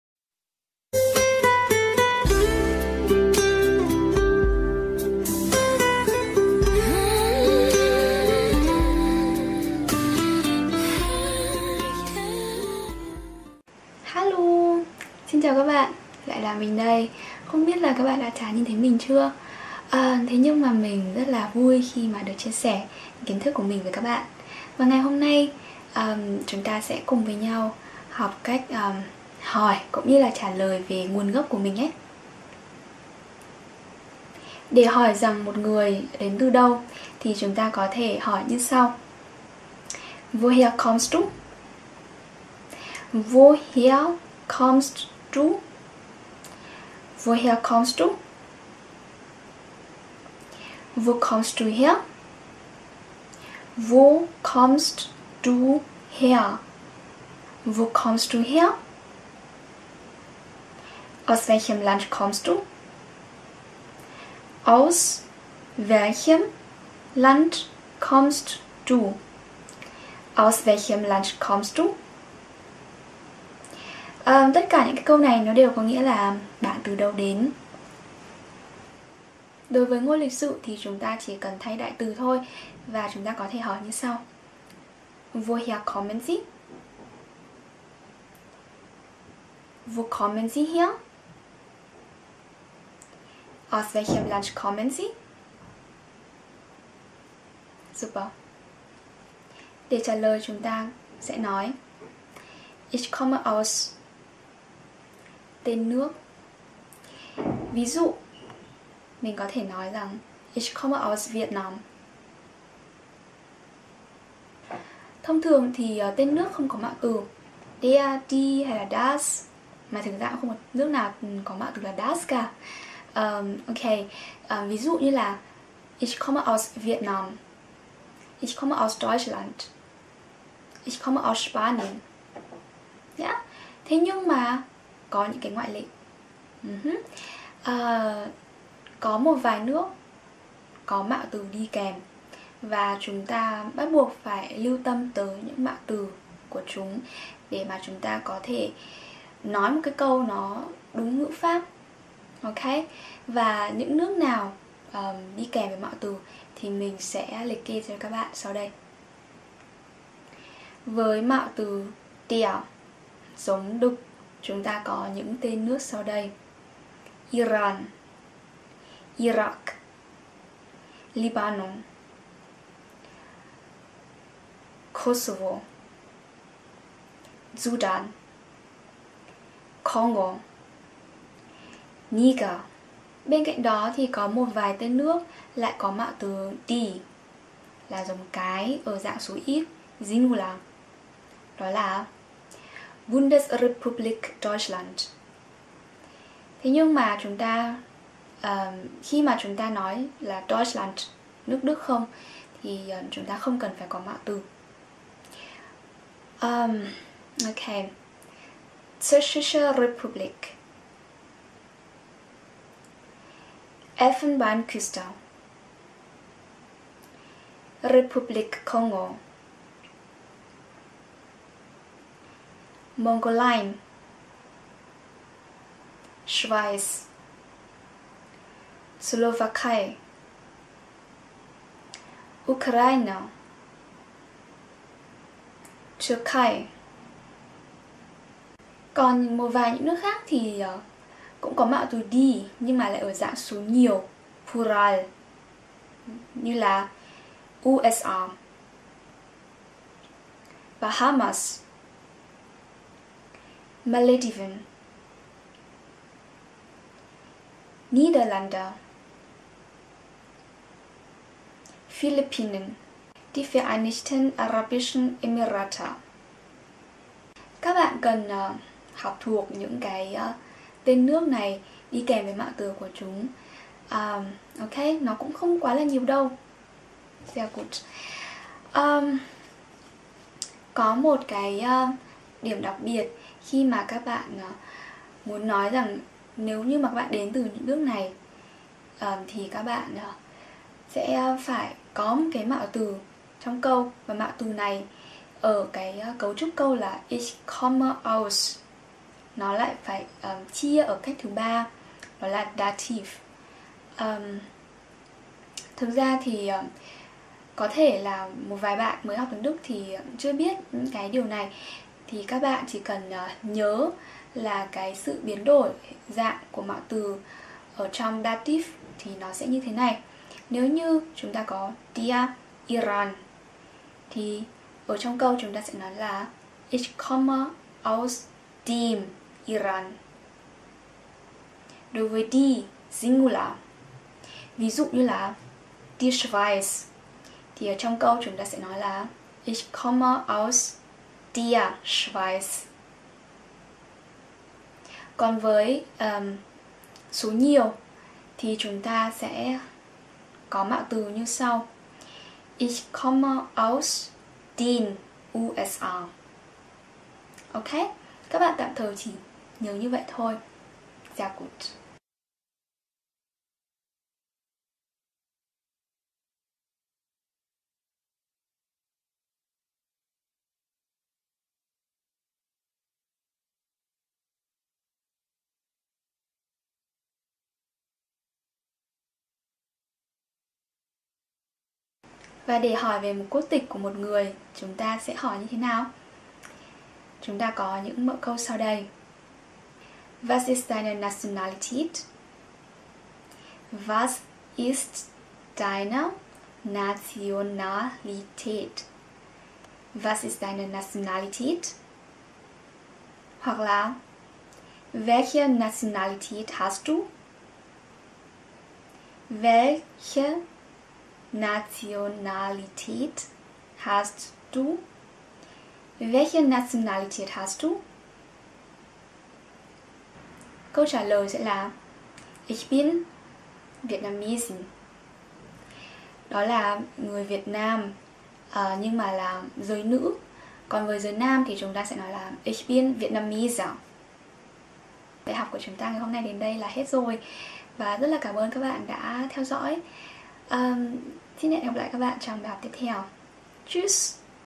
Lektion 5 - Giới thiệu về nơi xuất thân (Herkunft)
Trong bài này chúng ta sẽ học các mẫu câu để giới thiệu về nơi xuất thân của mình. Bên cạnh đó các bạn cần lưu ý một số tên nước có mạo từ (Artikel) đi kèm.

* Tài liệu tham khảo:
Danh sách tên nước và quốc tịch: